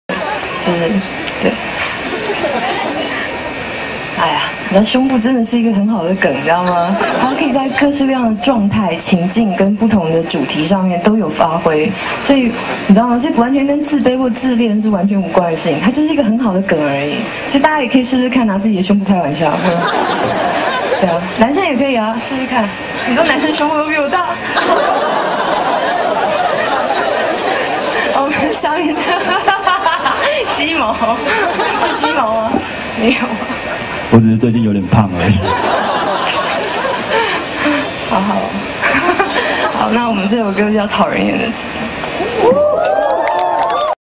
20090726張懸 城市行腳 台北 張語錄之三
20090726張懸在城市行腳於台北華山藝文中心時的張語錄之三